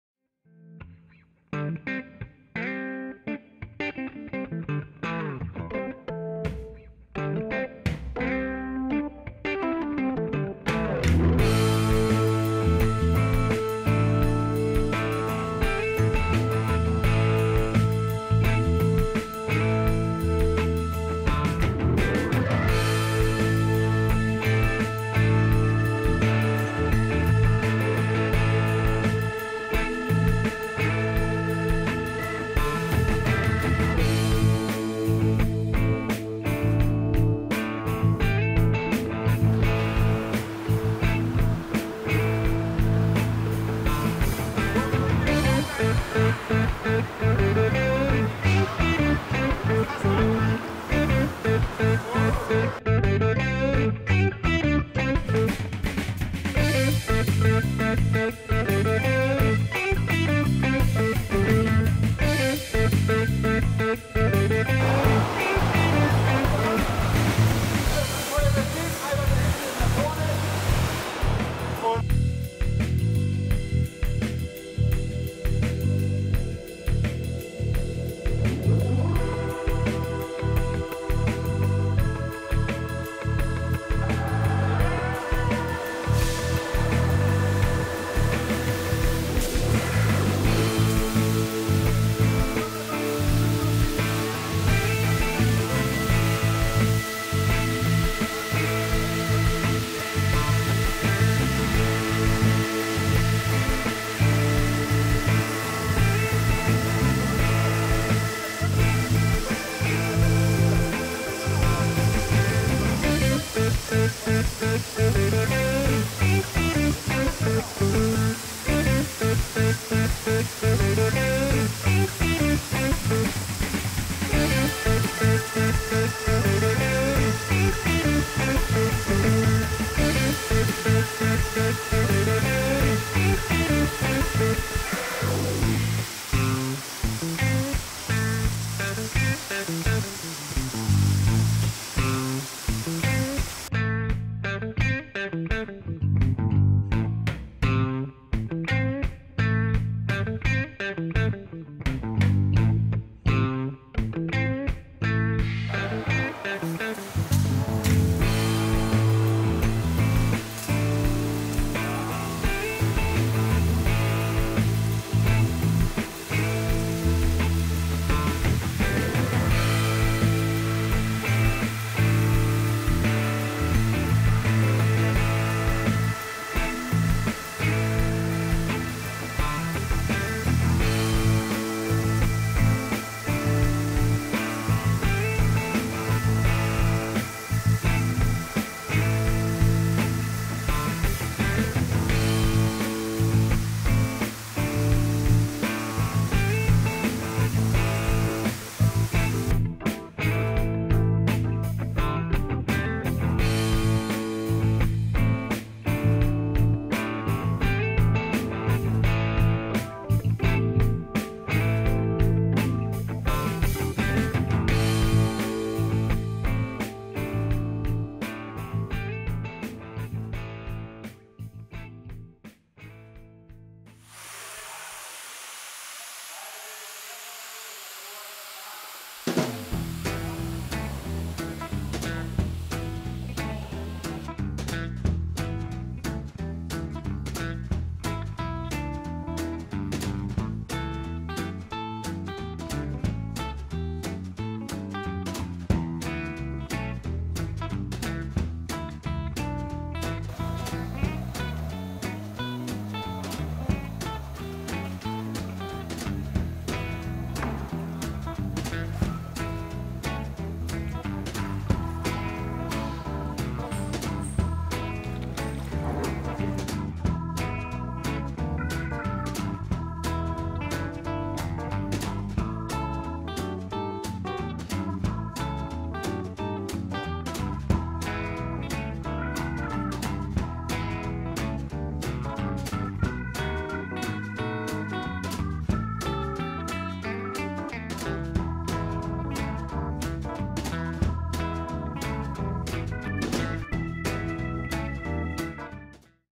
ISAF "Safety at Sea" Seminar
Video and pictures during the weekend "Safety at Sea" Seminar in November 2014 in order to fulfill the ISAF requirements for offshore regattas. Held at a marine base in Neustadt / Holstein in Germany, we were able to make use of the leak chamber, the fire hall and the wave pool for realistic exercises. These, combined with a lot of hands-on classroom material, made for an excellent course